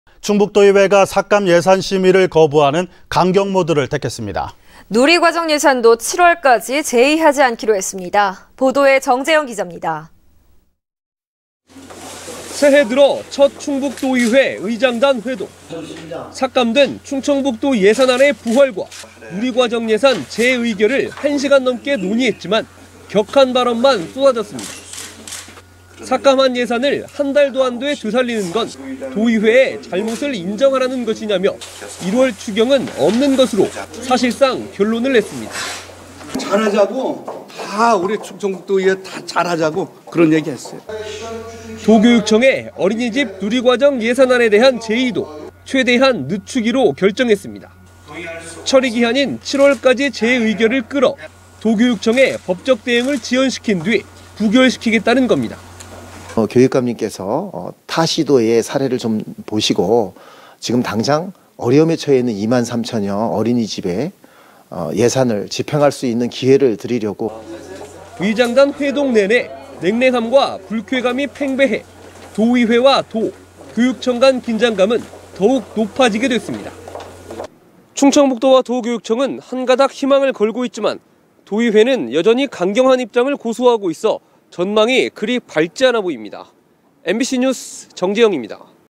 [청주MBC뉴스] "1월 추경 안받겠다"
◀앵커▶
 충청북도의회가 삭감 예산 심의를 거부하는
강경 모드를 택했습니다. 누리과정 예산도
7월까지 재의하지 않기로 했습니다.
 정재영 기자입니다.
           ◀ＥＮＤ▶

           ◀ＶＣＲ▶
 새해 들어 첫 충북도의회 의장단 회동.

 삭감된 충청북도 예산안의 부활과
누리과정 예산 재의결을 한 시간 넘게
논의했지만 격한 발언만 쏟아졌습니다.

 삭감한 예산을 한 달도 안돼 되살리라는 건
도의회에 잘못을 인정하라는 것이냐며
1월 추경은 없는 것으로 사실상 결론을
냈습니다.

 ◀ＩＮＴ▶이언구/충북도의회 의장
"잘하자고. 우리 충청북도의회 다 잘하자고
그런 얘기 했어요.

 도교육청의 어린이집 누리과정 예산안에 대한
재의도 최대한 늦추기로 결정했습니다.

 처리기한인 7월까지 재의결을 끌어
도교육청의 법적 대응을 지연시킨 뒤
부결시키겠다는 겁니다.

 ◀ＩＮＴ▶윤홍창/충북도의회 교육위원장
"교육감이 아이들 위해 예산집행할 시간주는것

 의장단 회동 내내 냉랭함과 불쾌감이
팽배해 도의회와 도,교육청간 긴장감은
더욱 높아지게 됐습니다.

(S/U)충청북도와 도교육청은 한 가닥 희망을
걸고있지만 도의회는 여전히 강경한 입장을
고수하고 있어 전망은 그리 밝지 않습니다.